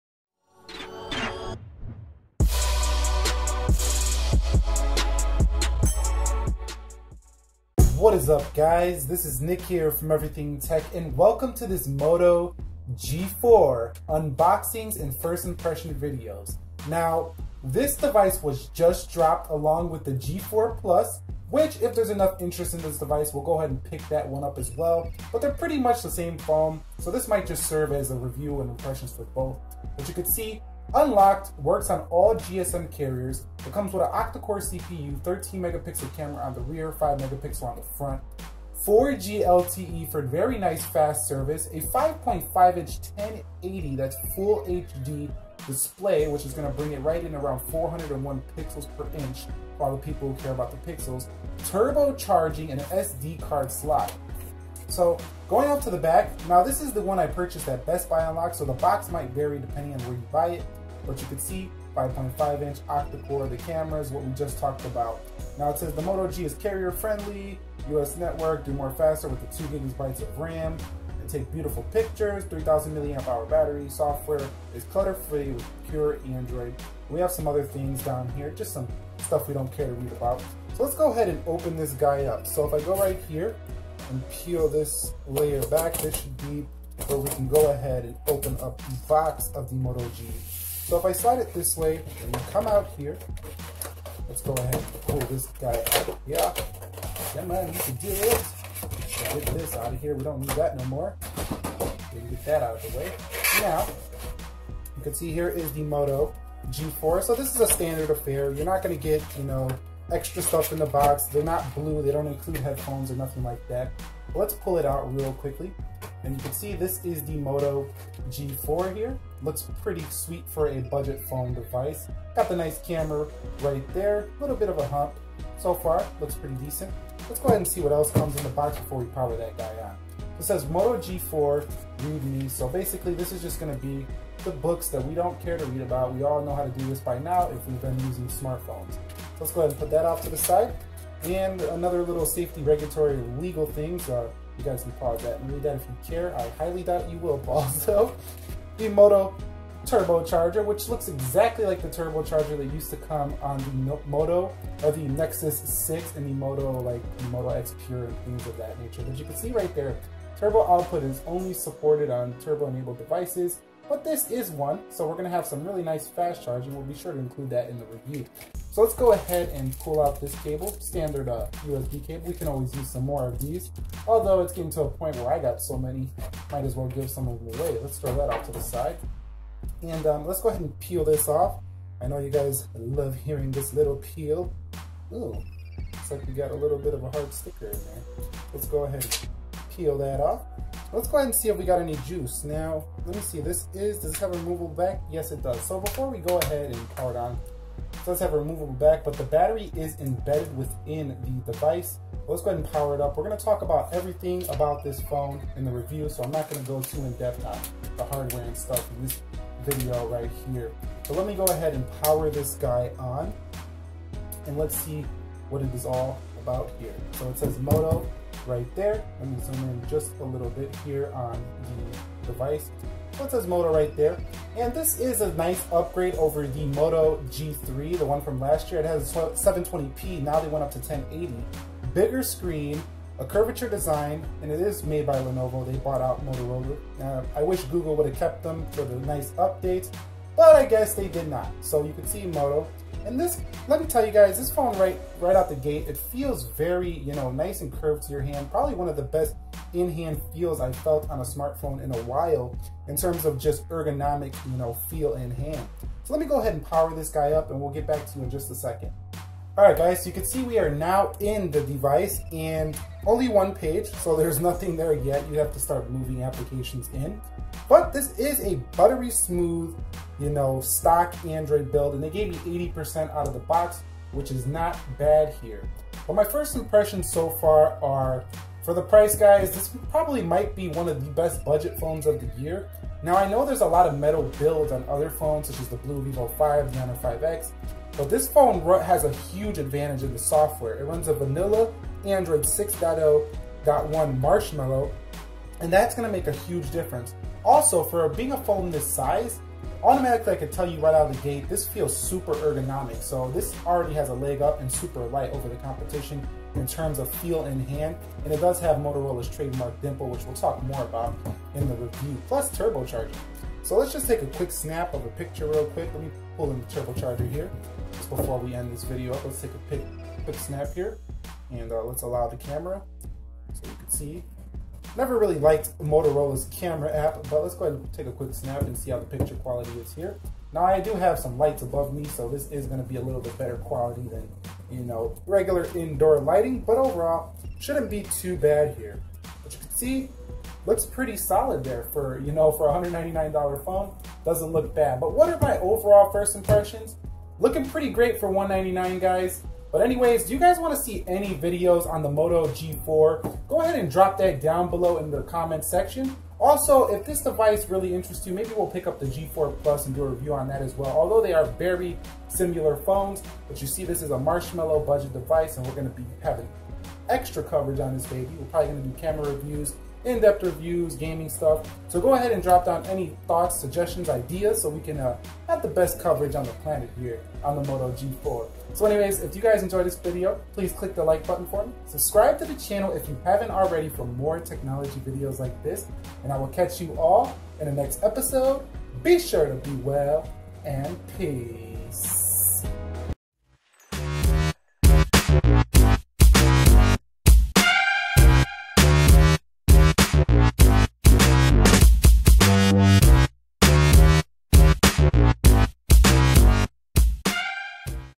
Moto G4 Unboxing: Curvy Android
Hello there everybody around the world, and welcome to this Moto G4 unboxing review video. In this video we will take a first look at the Moto G4 unlocked and see what it is all about. This video is a preparation to full review. The intentions of this video is to spur on a discussion and get your thoughts and questions about the Moto G4 before we go ahead and decide to make a video on the device.

Moto G4 Pricing and Information

Moto G4 Plus Pricing and Information

My YouTube Production Gear:

Canon SX530 HS
Samsung WB350F
Sunpak Tripod
Blue Snowball Microphone
Professional Lavlier Lapel Mic